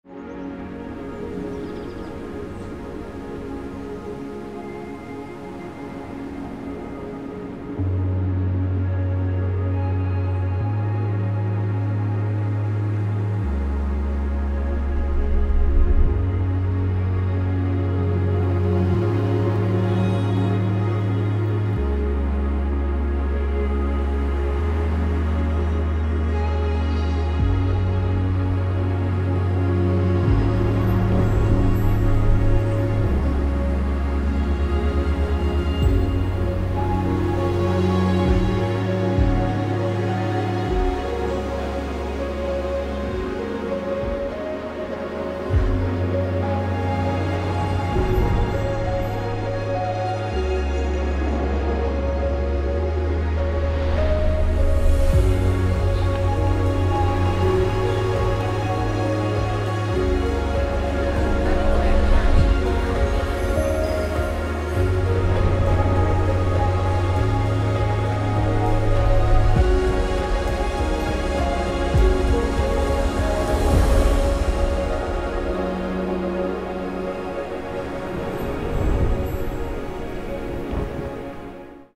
Endless Serenity: Chapter 1 | Rove Concepts Home Series
Immerse yourself in the enchanting ambiance of modern and sophisticated designs. Bathed in the gentle breeze and the sun's warm glow, this West Coast home complements its surroundings with luxurious silhouettes that echo the tones in nature.